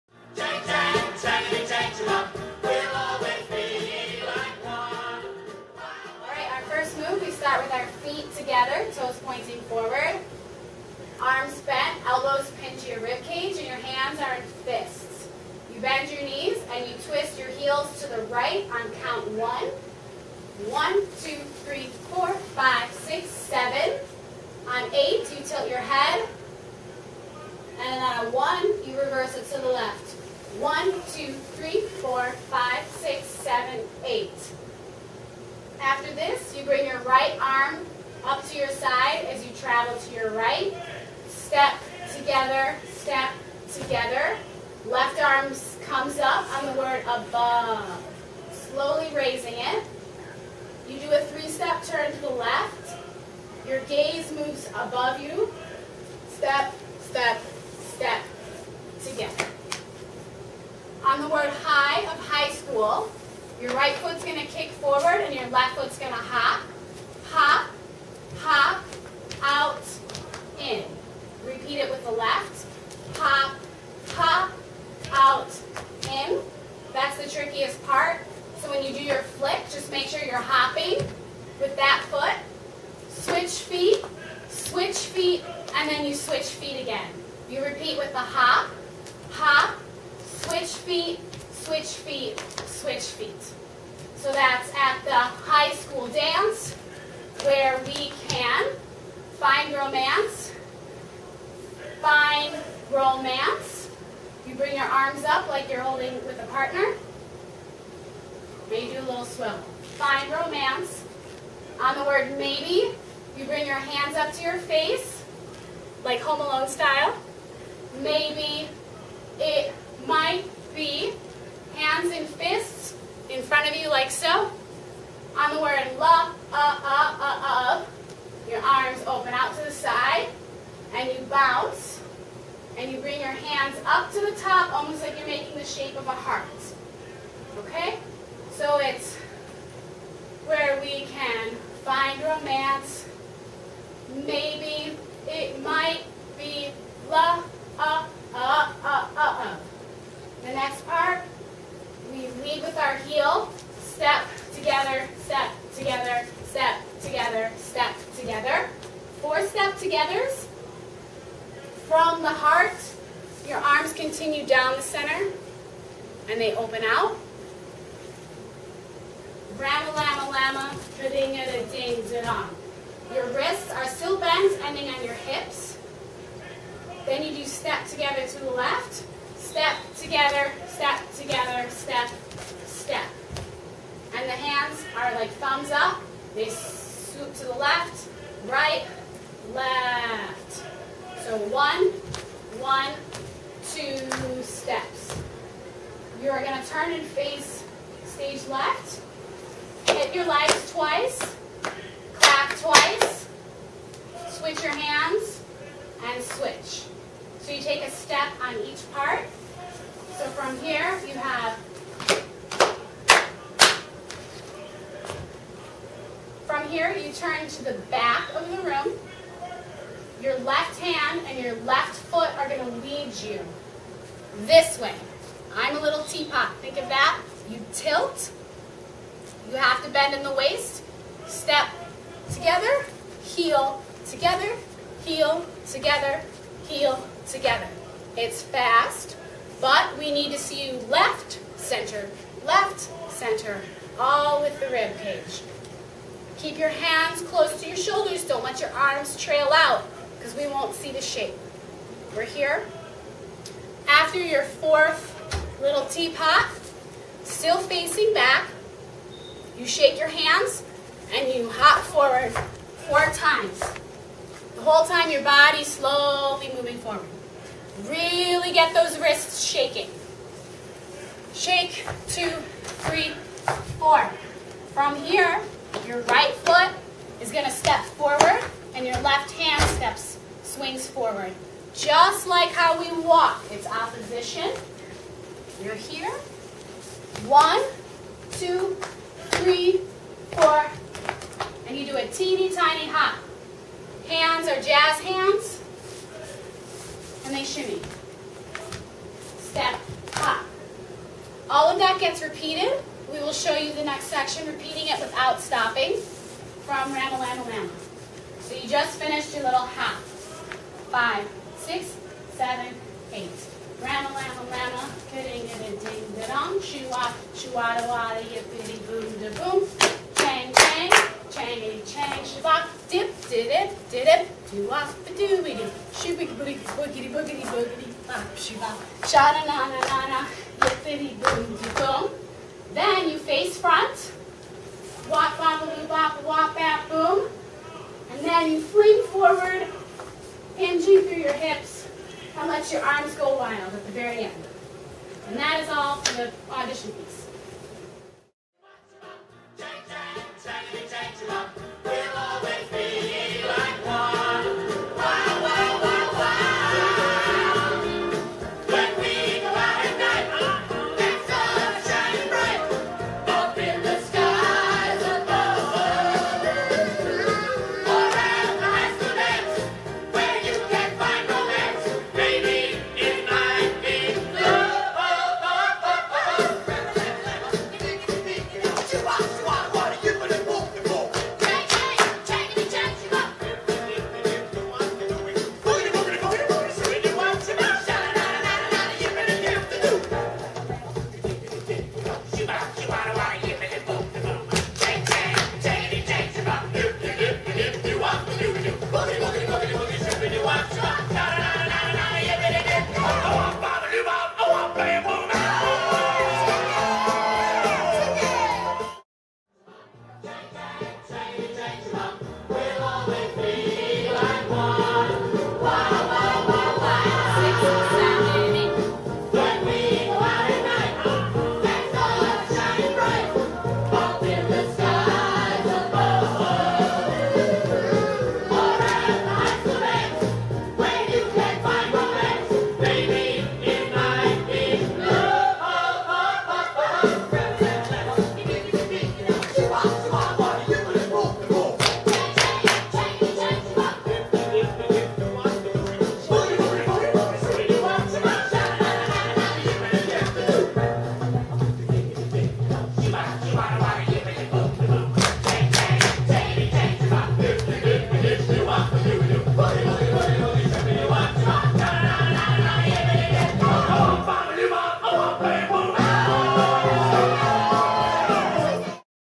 Grease Auditon Choreography.m4v
Audition choreography for BMS Station Middle School's spring musical, GREASE!

Also, congratulations to this year's Student Assistant Choreographers, (SACs,) Avery, Maddy and Nina. If you have questions or need clarification, they are here to help.